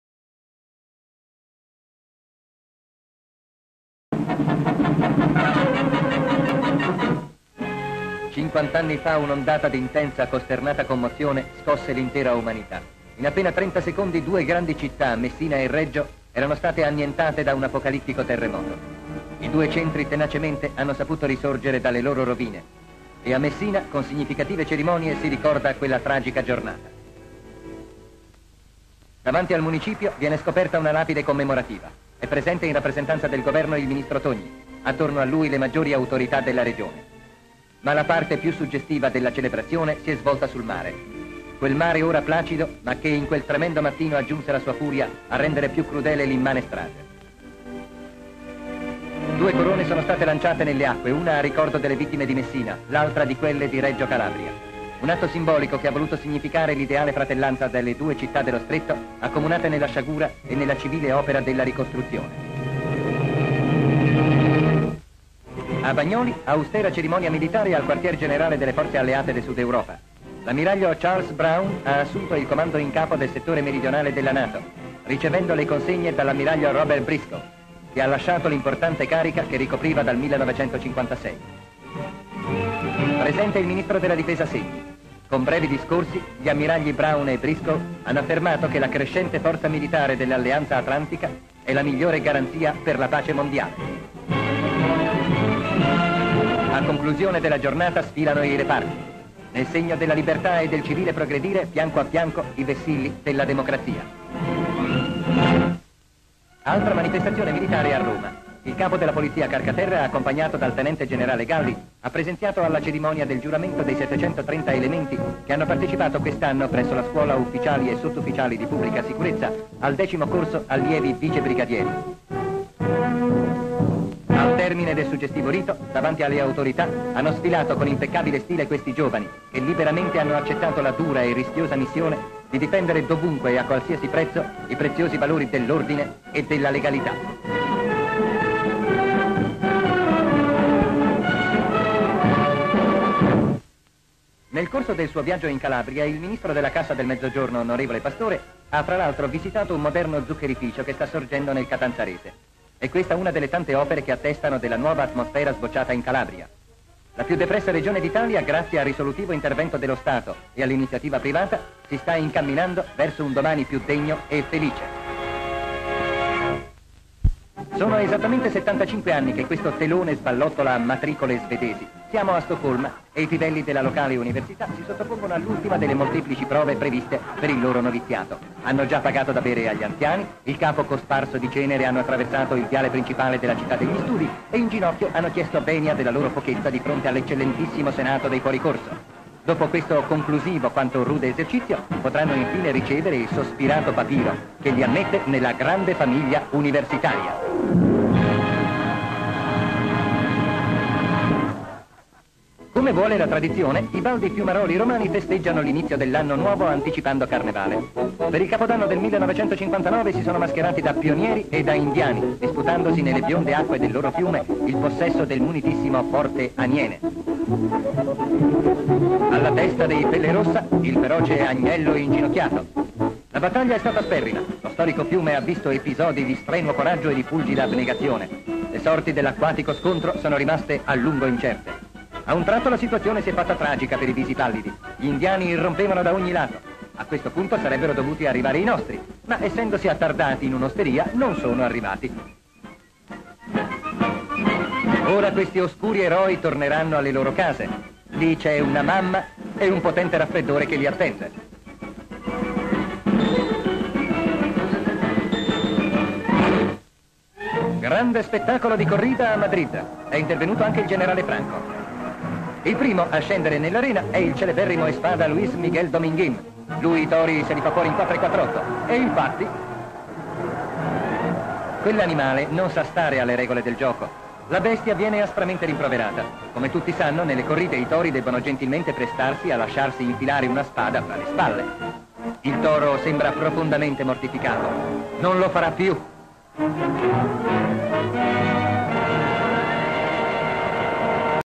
Cronaca con l'obbiettivo.
Messina ricorda il terremoto del 1909.
    Nominato il nuovo comandante delle Forze Alleate del Sud Europa
    La cerimonia di giuramento degli allievi della Scuola di ufficiali e sottufficiali di Pubblica sicurezza a Roma
    Visita del ministro Pastore presso il nuovo zuccherificio che sta sorgendo nel catanzarese
    Divertimenti della matricole svedesi a Stoccolma
    Gare tra barcaioli romani
    Corrida a Madrid, presenziata da Franco.
   Descrizione sequenze:Messina ; titoli dei giornali sul terremoto ; panoramica di Messina ; una lapide commemorativa viene scoperta ; Togni stringe la mano ad autorità locali, un sindaco nel gruppo di personalità presenti alla cerimonia ; folla su lungomare ; una colonna sul molo ; su una nave, i marinai sull'attenti accanto a corona di fiori ; si incontrano due navi in mare, la corona di fiori viene gettata in acqua ; un elicottero sorvola messina ; le autorità sul ponte della nave muovono il braccio in segno di saluto ; la scia della nave. ; Bagnoli ; autorità militari schierate nel cortile del quartiere generale ; autorità militari e religiose sul palco ; un membro della marina saluta Brown sull'attenti ; militari sorreggono bandiere che sventolano ; Brown passa in rivista i diversi reparti presenti ; Segni, accanto ad ammiraglio, assiste alla cerimonia ; i discorsi degli ammiraglio si susseguono sul palco ; i carabinieri e i diversi reparti sfilano ; Roma ; Carcaterra, accompagnato dal generale Galli, passa in rivista gli allievi di polizia schierati nel cortile della scuola ; sul palco, un allievo legge il giuramento, appone firma su registro ; Carcaterra stringe la mano all'allievo ; gli allievi sfilano davanti al palco delle autorità ; le autorità presenziano ; gli allievi schierati, un gruppo di allievi sfila con la bandiera ; Calabria ; una macchina percorre una strada lungo area di impianti ; il cartello della Cassa per il mezzogiorno ; Pastore, con autorità al seguito, davanti alle strutture dello zuccherificio ; il gruppo di autorità si allontana dall'impianto ; una bimba dona un mazzo di fiori al ministro ; i presenti applaudono ; una macchina si allontana dall'impianto ; Stoccolma ; giovani matricole si lanciano su tendono sorretto da studenti universitari ; gli studenti agitano il tendone, le matricole saltano sul tendone ; Roma ; giovani con uniformi da soldati americani ottocenteschi scendono la scalinata lungo il tevere ; giovani vestiti da cow boys e indiani sulla banchina ; i giovani poggiano in acqua una canoa coperta ; un ragazzo vestito da indiano finge di cavalcare un cavallo ; i giovani a bordo di canoe gareggiano nel fiume ; un ragazzo con tuta da sub esce dall'acqua ; Madrid ; i toreri entrano nell'arena ; Franco si affaccia dal balcone ; l'arena gremita di pubblico ; il toro colpisce il torero, lo getta a terra ; uomini sopraggiungono nell'arena per bloccare il toro, che si libera ; il toro rincorre il torero ; il torero calma il toro, gli mette una mano sulla testa. . ;